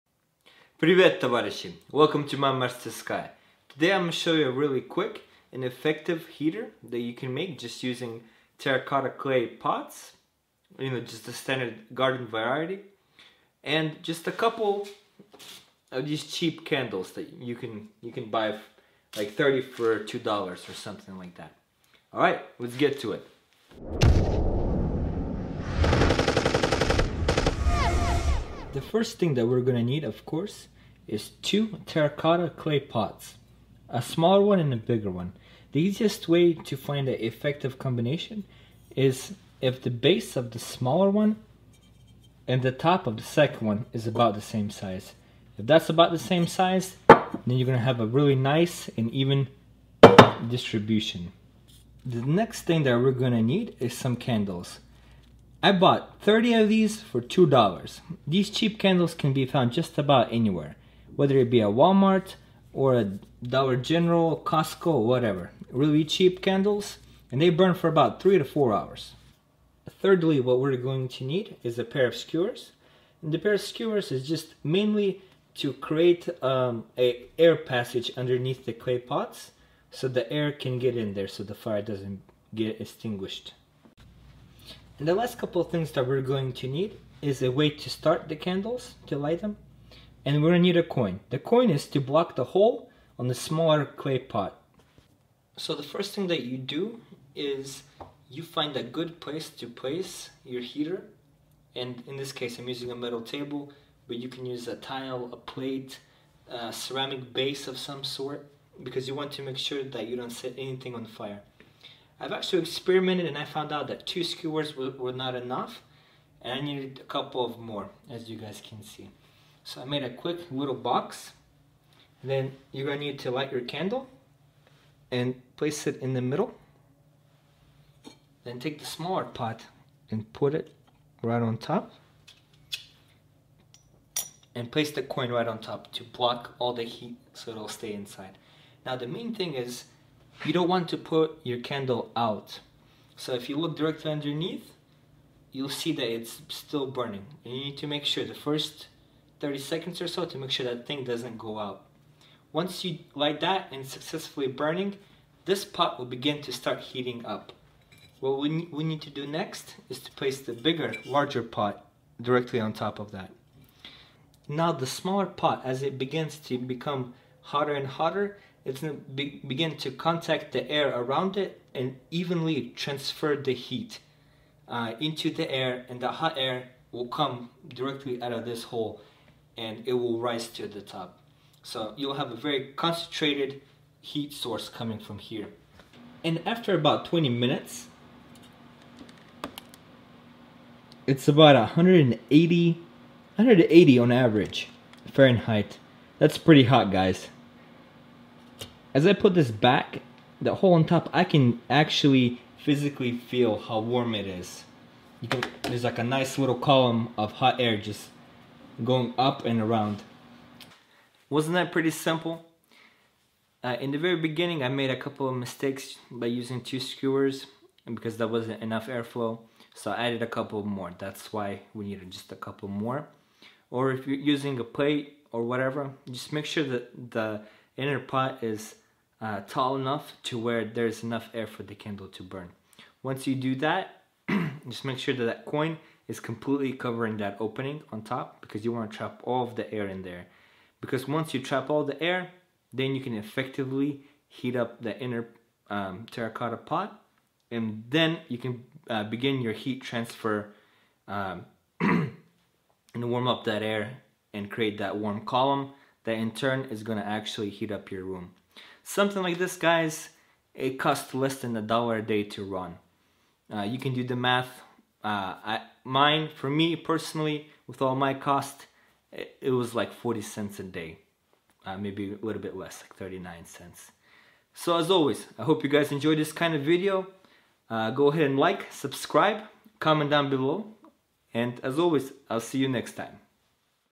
DIY Clay Flower Pot Candle Heater
Super easy to do, all you need is two clay pots ,one bigger and one smaller and a short candle to heat up the small pot. the heat off the small pot will warm up the air between the two pot and create a jet of warm air that in turn will heat your room. go out there and experiment for yourself and see if you can make a better design and thanks for watching!